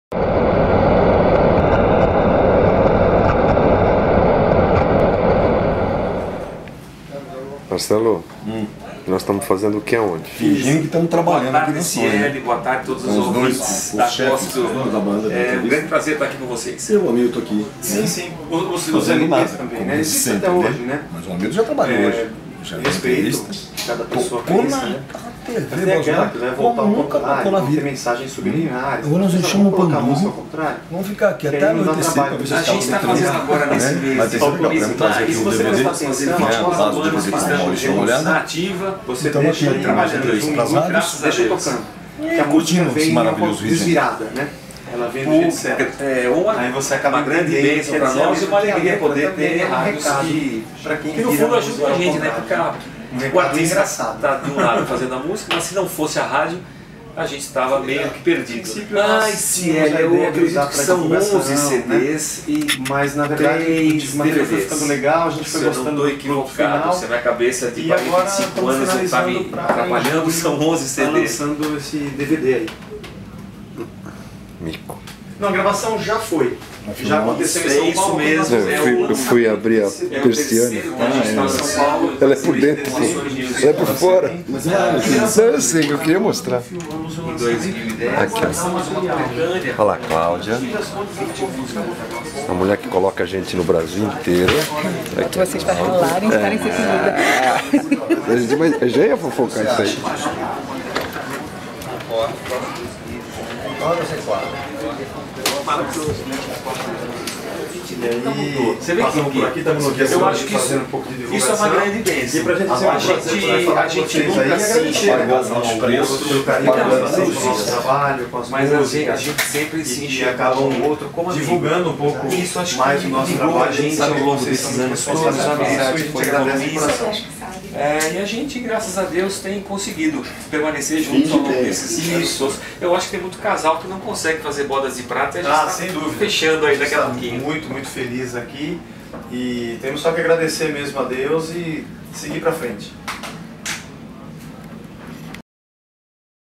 Resgate na Sony Music
Visita do Resgate nos escritórios da Sony Music Gospel no RJ em março 2013.